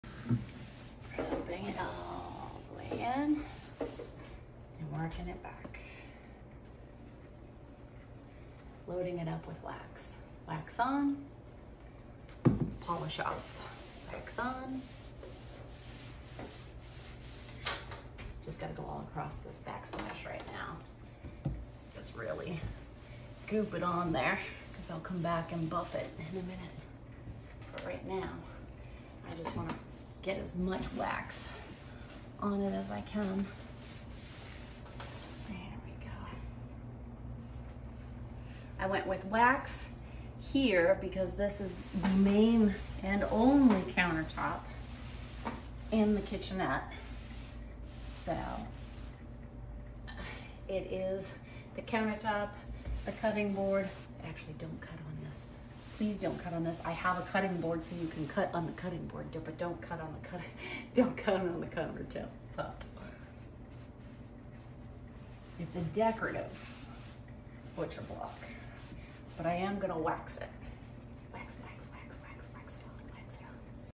Wax on then polish off
Waxing the walnut butcher block “Make it with Mary Construction”